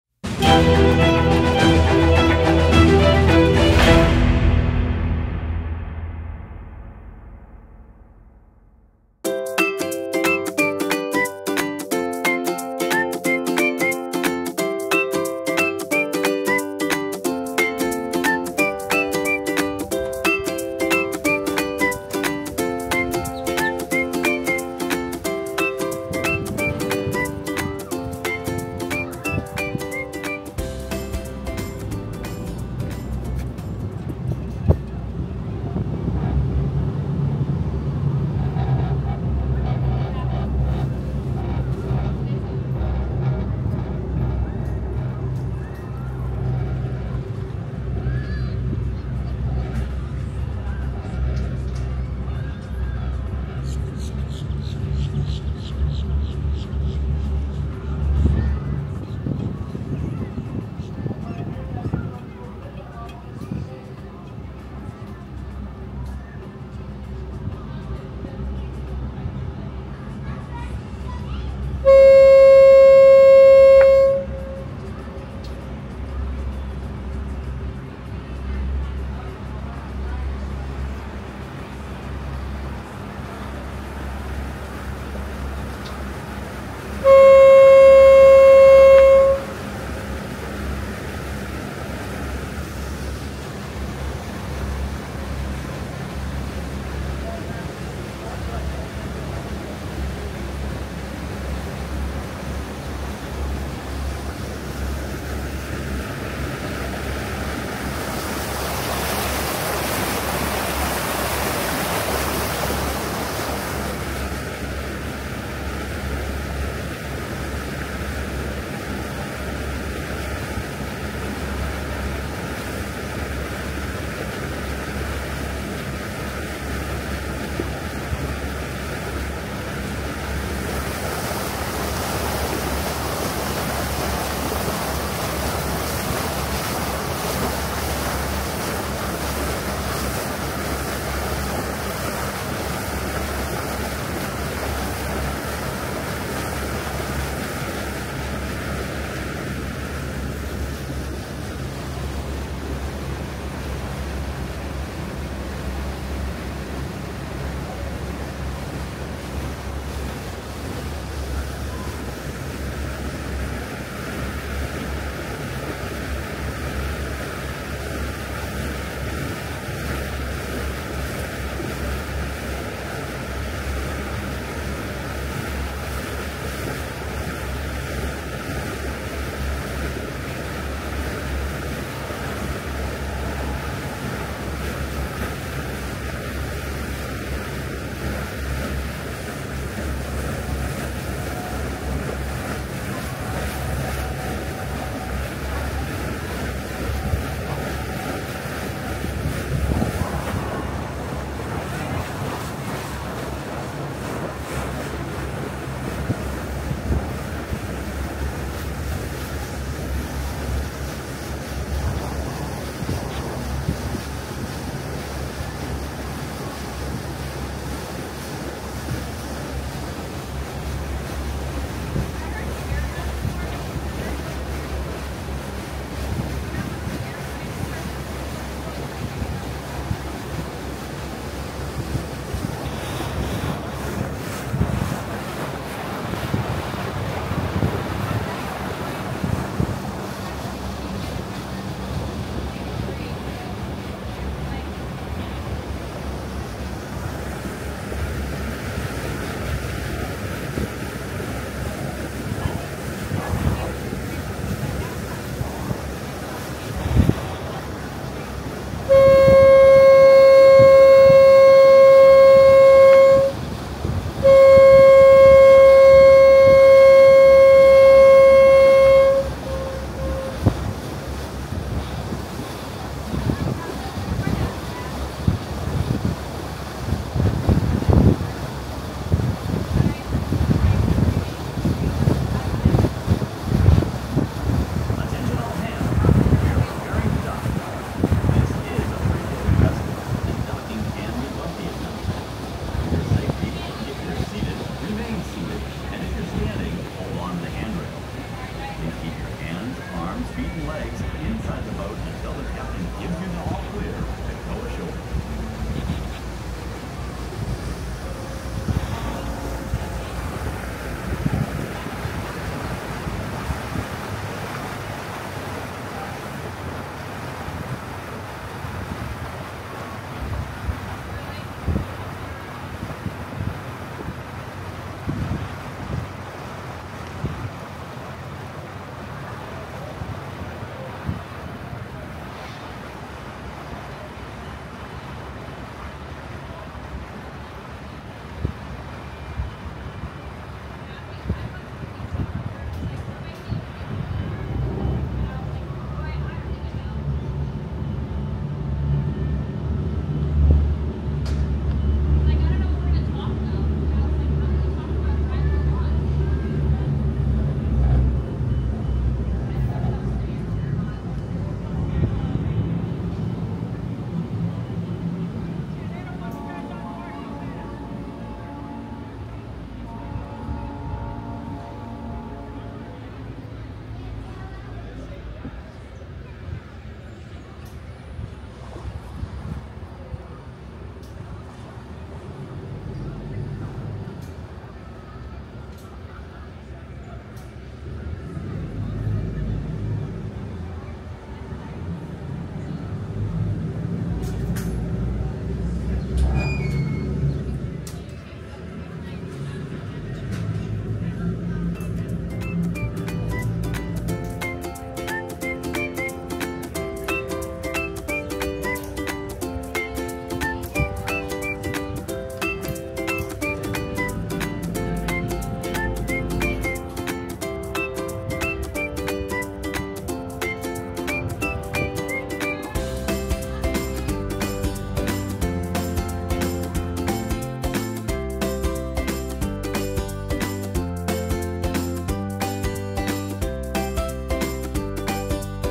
Water Taxi Journey from Polynesian Resort to Magic Kingdom | Seven Seas Lagoon Adventure in 4K
Join us on a magical water taxi journey across the breathtaking Seven Seas Lagoon, starting from the enchanting Polynesian Resort and heading towards the iconic Magic Kingdom! 🌊✨

In this video, you'll experience stunning lagoon views, catch glimpses of Cinderella's Castle from a unique perspective, and enjoy the serene beauty of Disney's waterways. This scenic boat ride offers a relaxing and picturesque way to travel between these iconic Disney destinations. Whether you're planning your next Disney trip or simply reminiscing about your last visit, this journey is sure to fill you with Disney magic and wanderlust.